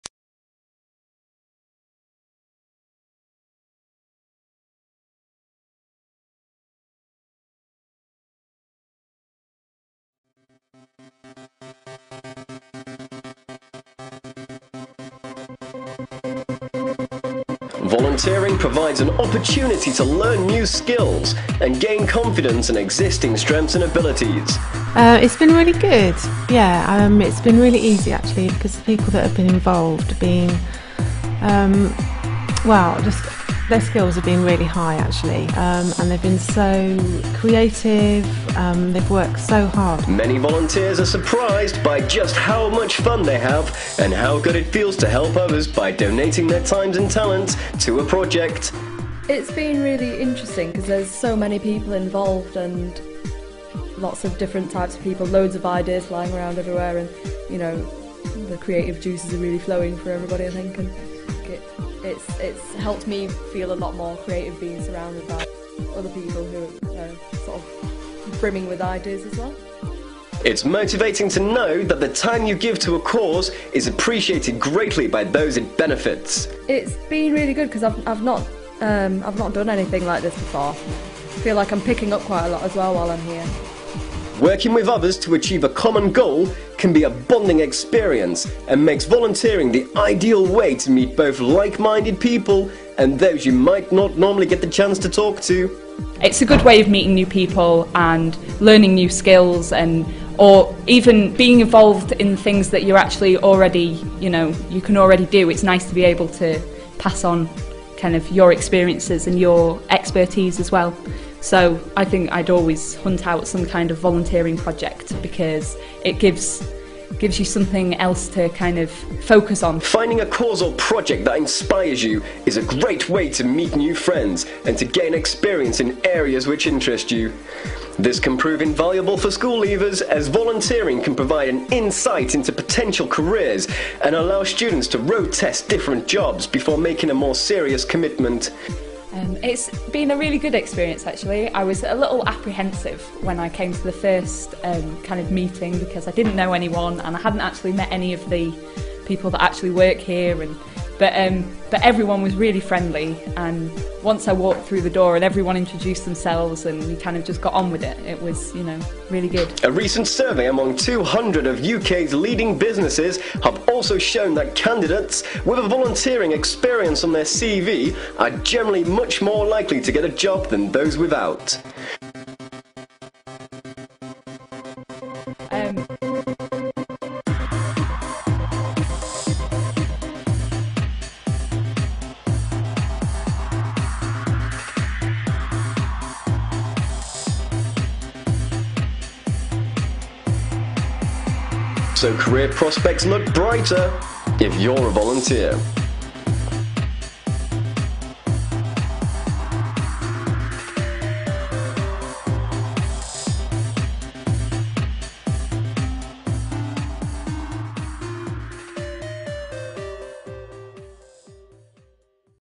Why volunteer
Animated video on the benifits of volunteering.